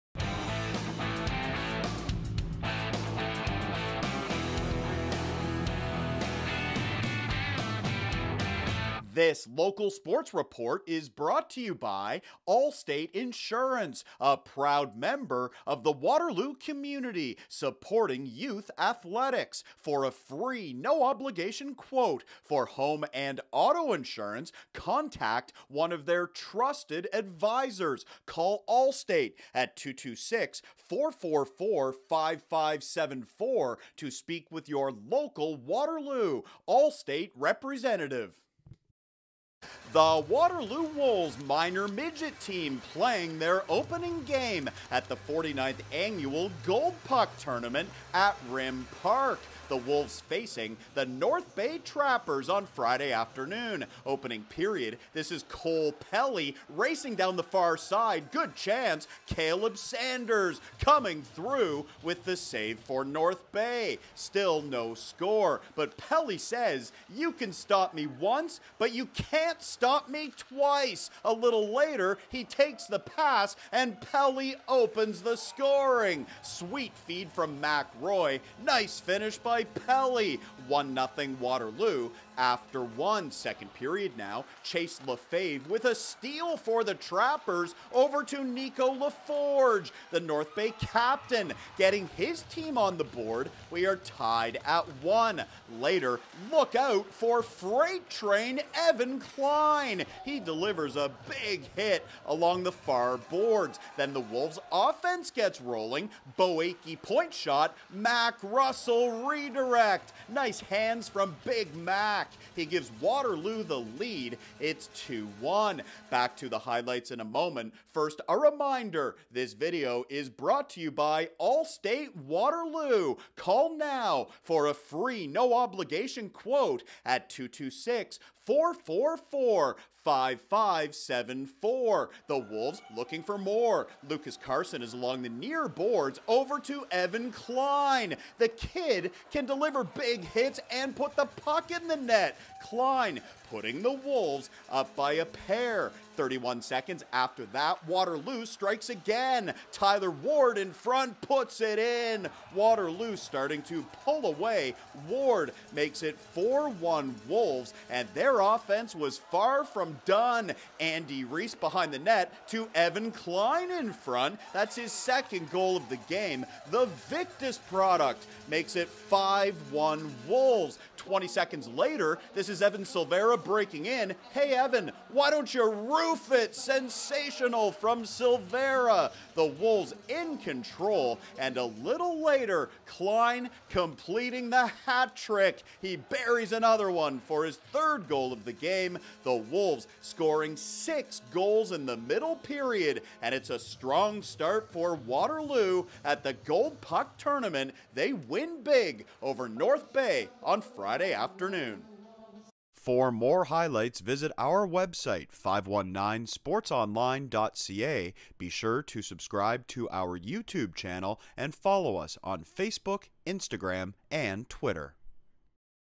Minor Midget (Gold Puck) - North Bay Trappers vs Waterloo Wolves
Highlights from Friday December 13th between the Trappers and Wolves at the Gold Puck tournament at RIM Park.